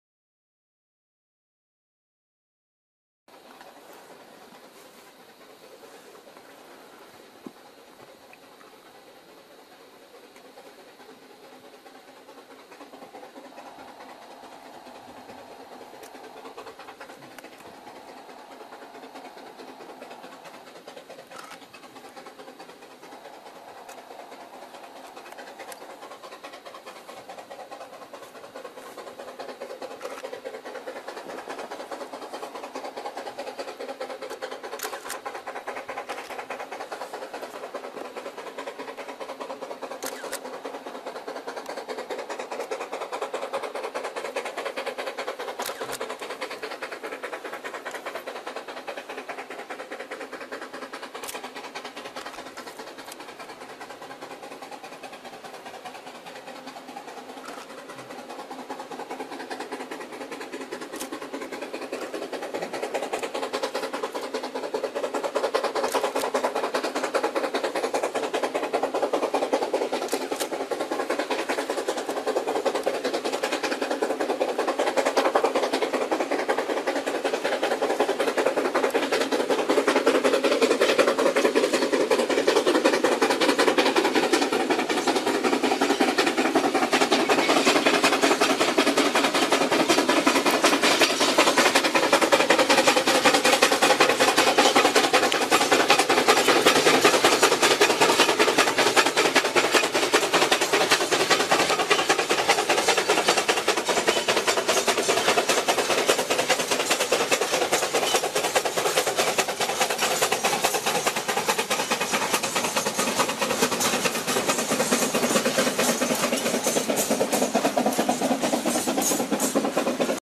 Steam of Weihe Forest Railway China(Feb.2002) 4 中国・葦河森林鉄道の蒸気機関車（2002年2月）4
2002年2月撮影。中国・葦河森林鉄道のC2です。平林ー双峰間。　BetweenPinglin and Shuangfeng.